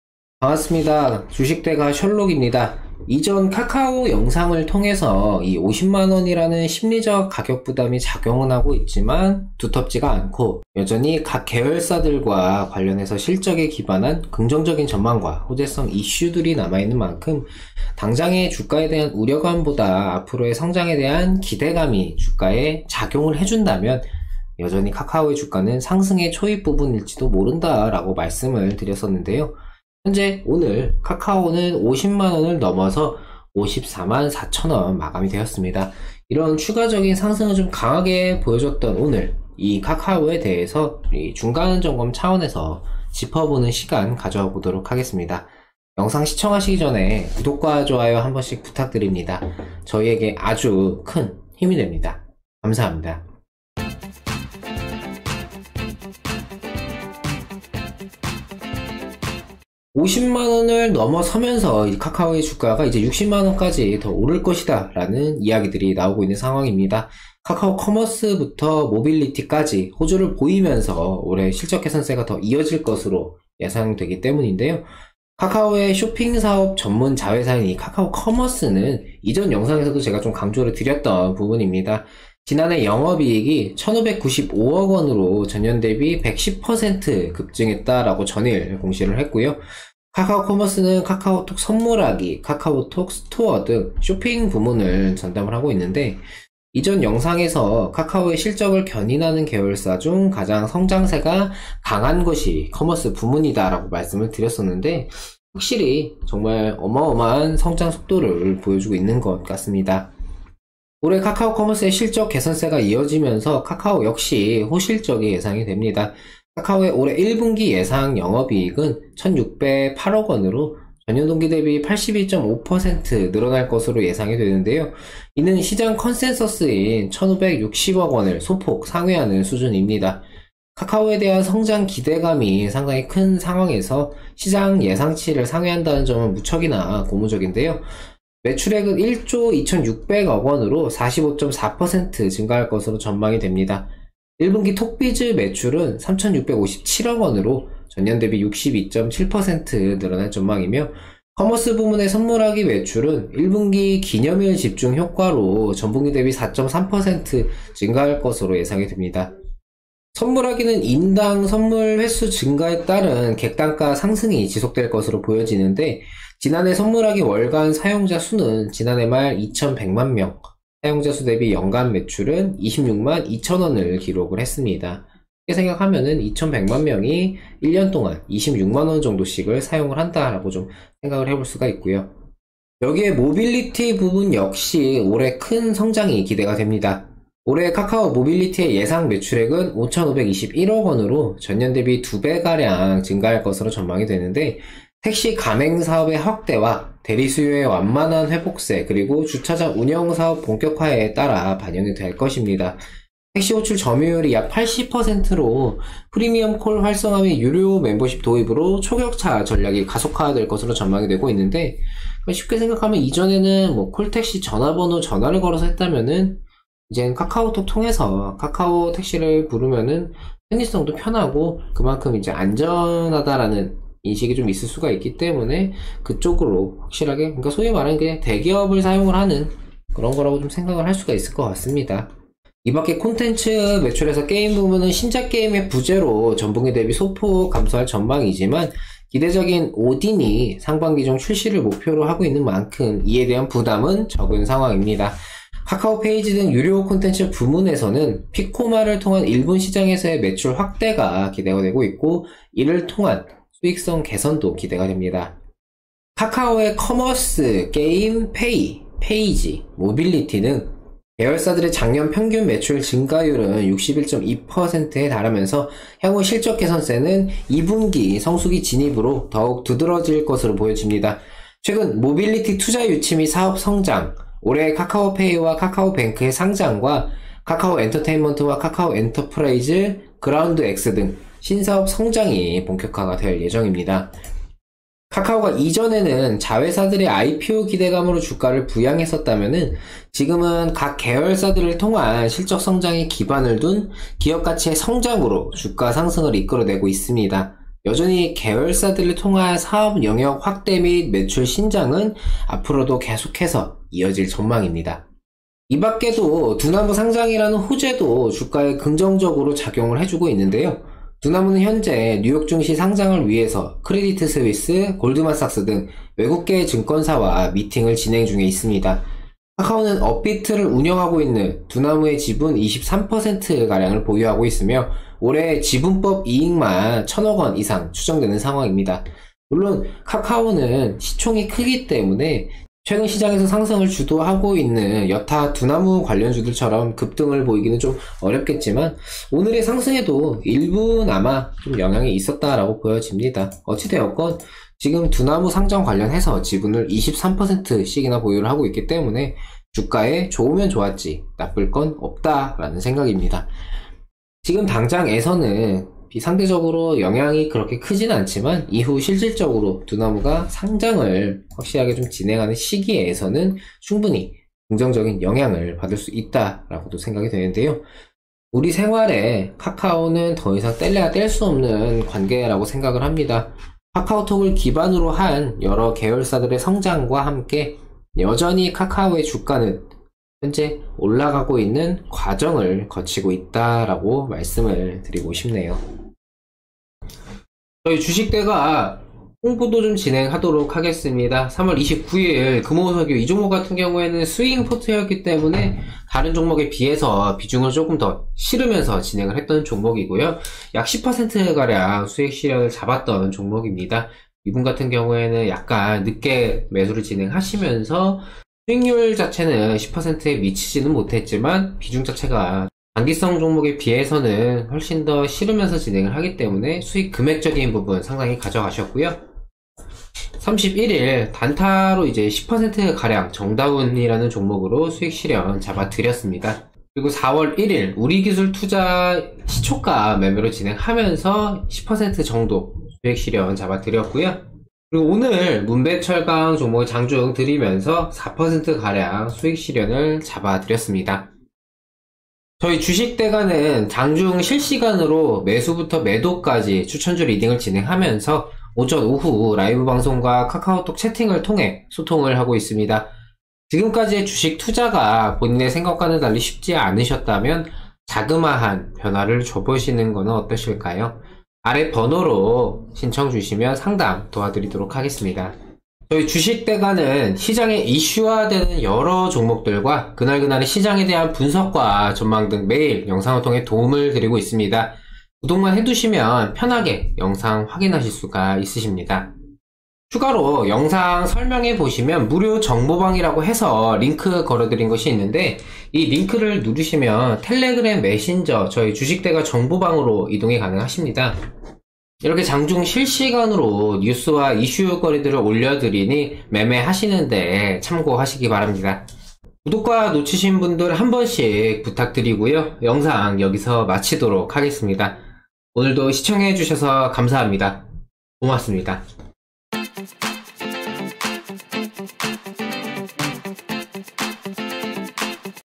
카카오 - 좋은 건 알겠는데.. 지금 사도 되나? 액면분할 하면 주가는?
방송시간  ★오전8시 45분 ★오후 2시 50분